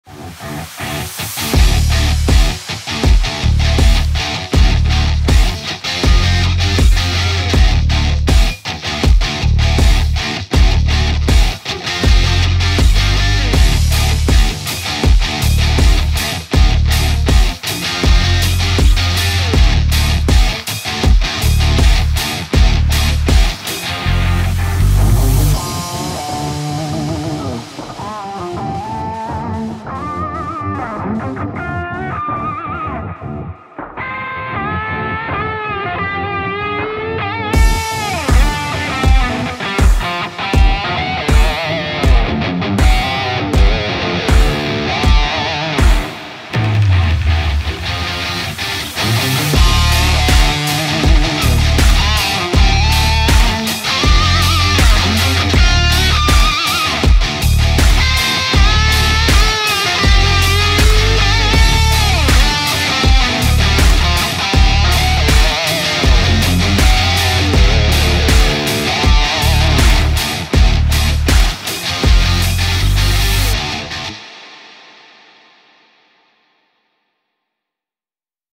URBANTRIALSHOW Structure #3 VAN - Remorque Julien PERRET
Show moto trial sur remorque VAN 2T5
sow cles en main.